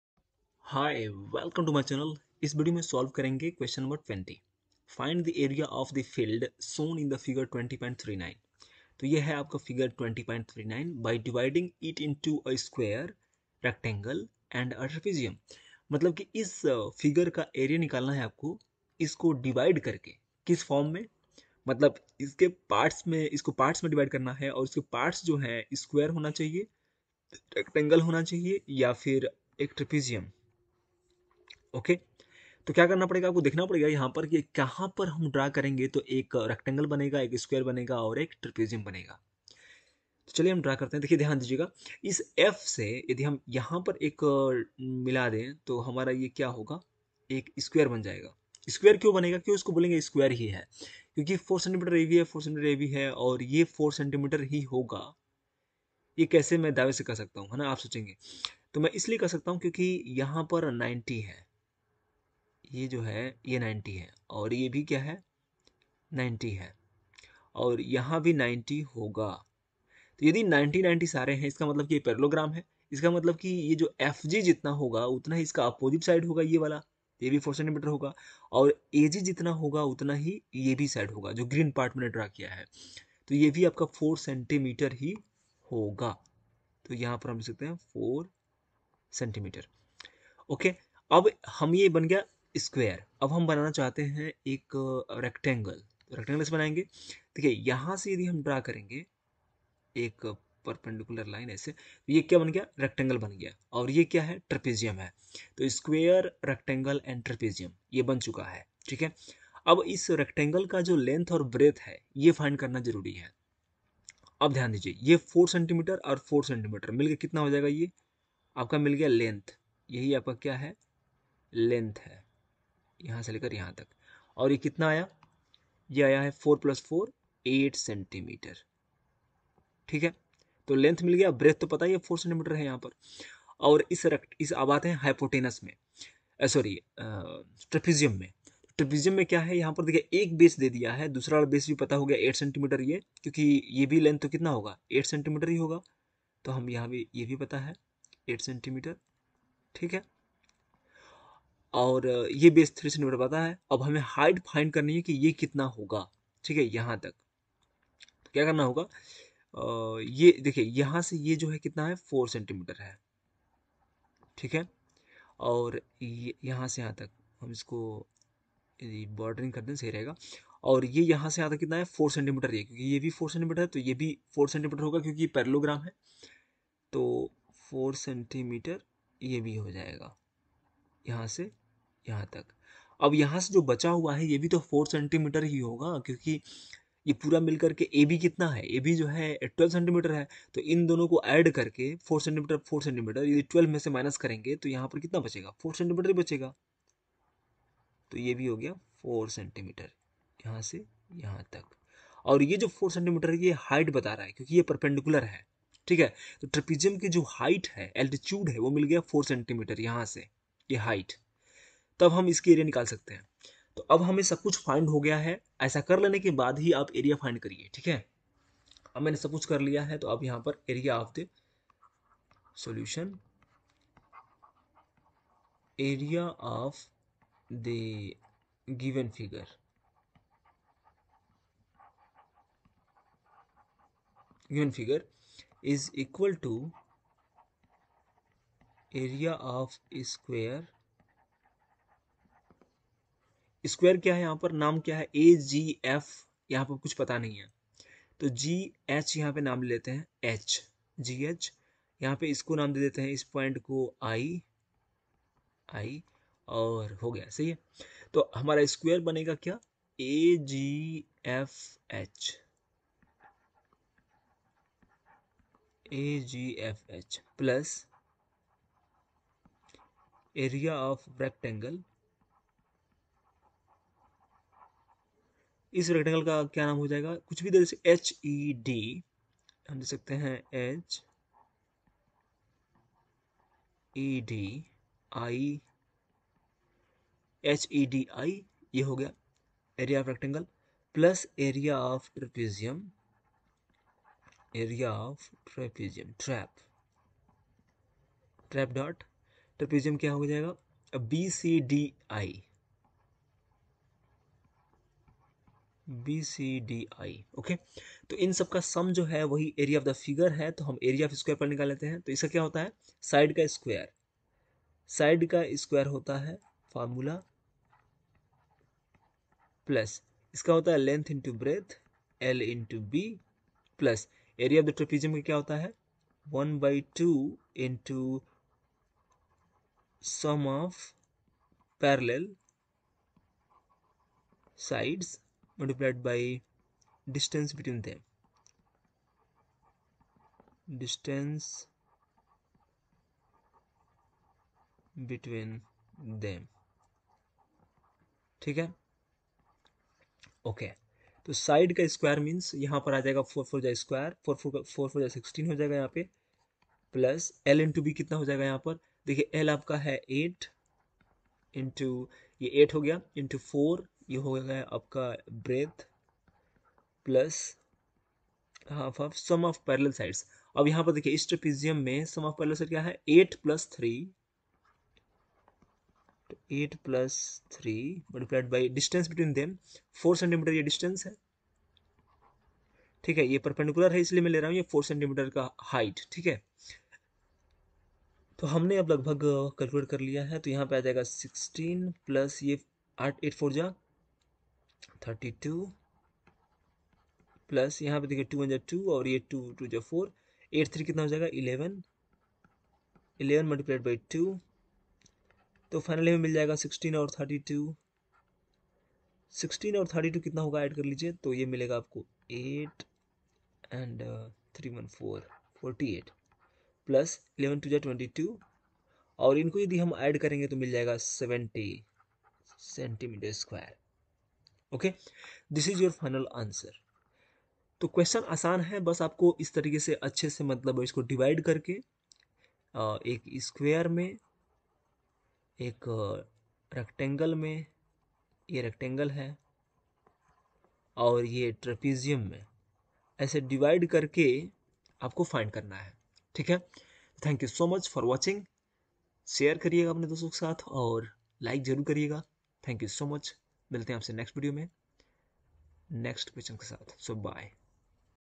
Q.20 - EXERCISE 20.2 - Chapter 20 Mensuration I Area of a Trapezium and a Polygon R D SHARMA CLASS 8
20. Find the area of the field shown in Fig. 20.39 by dividing it into a square, a rectangle and a trapezium.
Chapter 20 Mensuration I Area of a Trapezium and a Polygon R D sharma Class 8 Mathematics